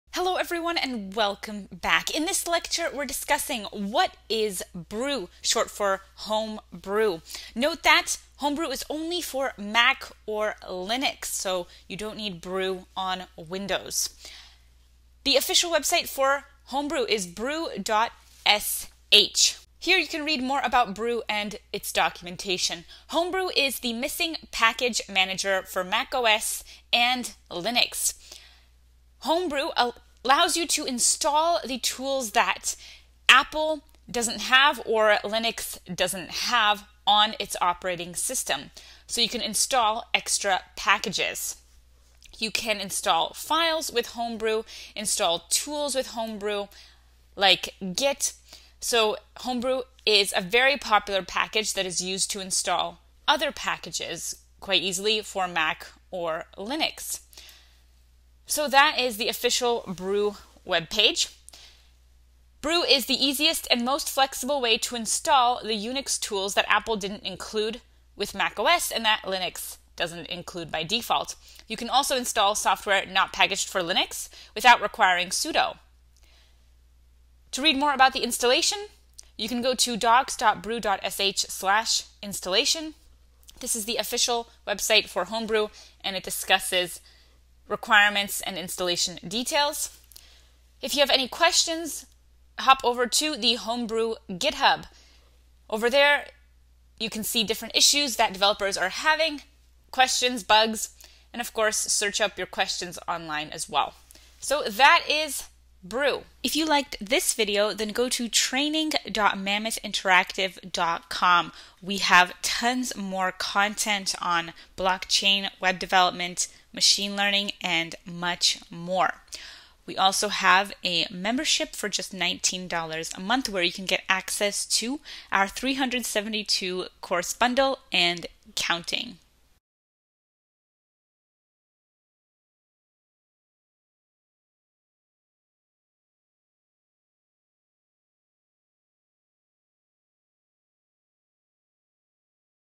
Homebrew EXPLAINED *Must Know for Mac/Linux Developers*
Learn Homebrew, the easiest and most flexible way to install the UNIX tools Apple didn’t include with macOS. Brew can also install software not packaged for your Linux distribution to your home directory without requiring sudo.